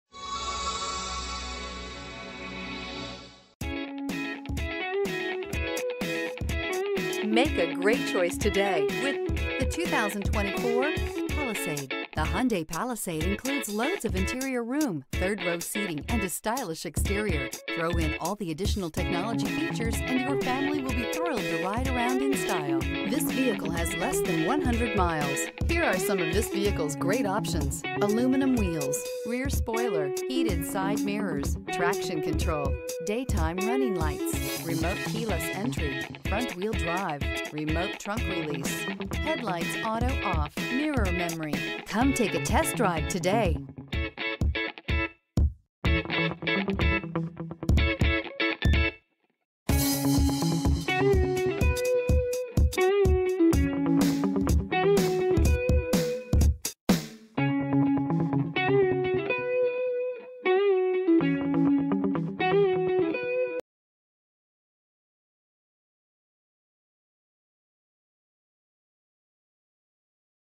2024 Hyundai Palisade P2384
2024 Hyundai Palisade SEL

Typhoon Silver 2024 Hyundai Palisade SEL FWD 8-Speed Automatic with SHIFTRONIC V6brbrRecent Arrival! 19/26 City/Highway MPG